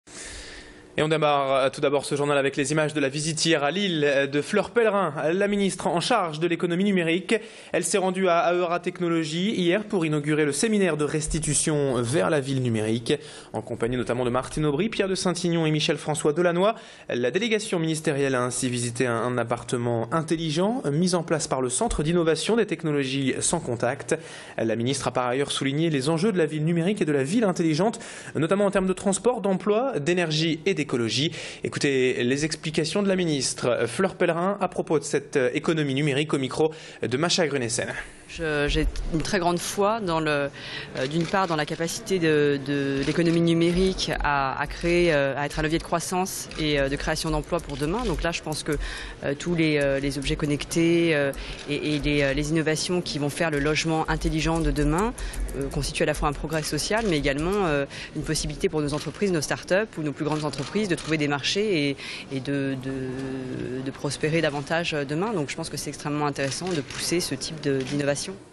Fleur Pellerin vers la ville numérique à Lille !
Fleur Pellerin, ministre de l'économie numérique, était à Lille pour ouvrir le séminaire de restitution de "Vers La Ville Numérique"

Sites webs/blogs, copiez cette vidéo avec le bouton "Intégrer" !